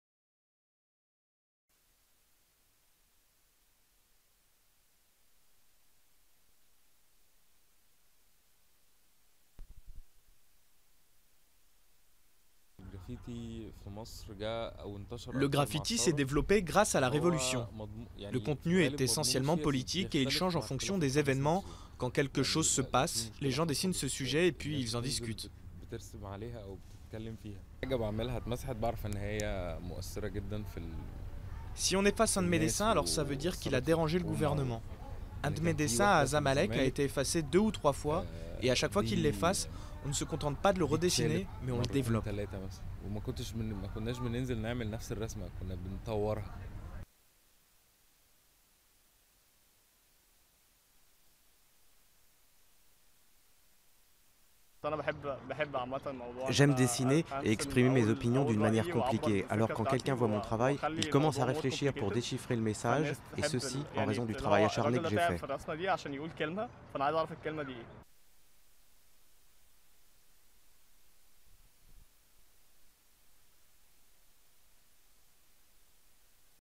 Au Caire, les graffitis racontent une révolution inachevée
Dans l'Egypte qui marque vendredi 25 janvier le deuxième anniversaire du début de la révolte qui fit chuter Hosni Moubarak, la peinture de rue est devenue le refuge de l'information et de l'art alternatifs. Durée: 01:16.